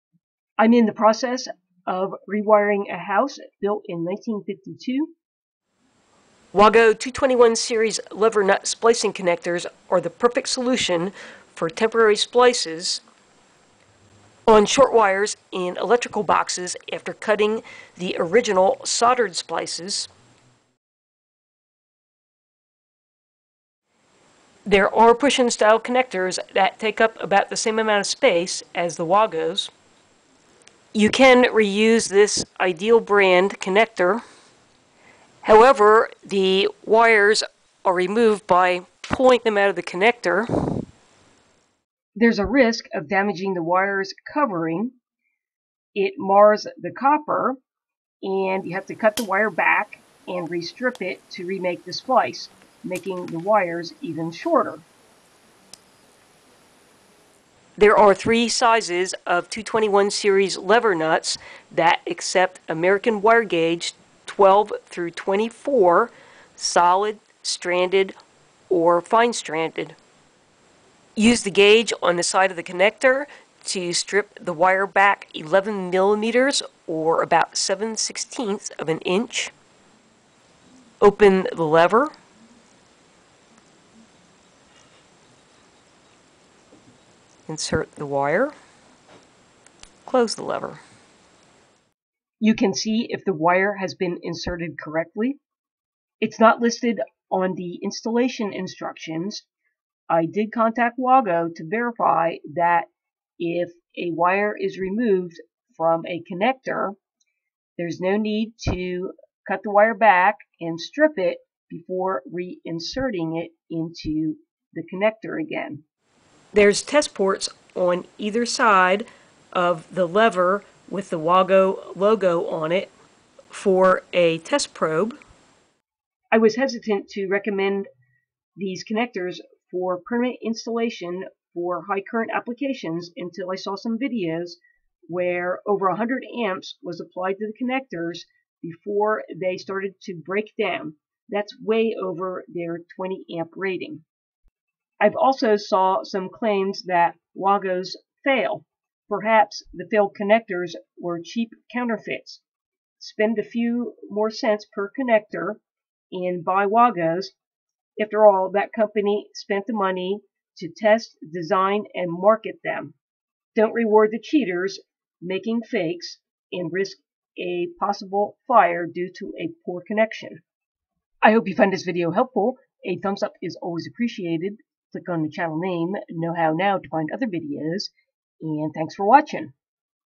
How to Use Wago Connectors - 221 Series Lever Nuts
Using Wago wire connectors for wires that are too short for wire nuts or for temporary splices are two advantages over wire nuts or push in connectors.  Wago wire nuts are good for DIY electricians because it's hard to make a poor connection with them.

Initially, I thought Wago wire connectors might be best used on low amp connections.  I saw some tests where over 100 amps were put through the connector and they held up.  They are rated for 20 amps.

I like to use these connectors on wires that are cut short,  making it difficult to use a wire nut.  I prefer them over push in connectors because the lever can be opened, the wire removed and then re inserted without having to cut an already short wire.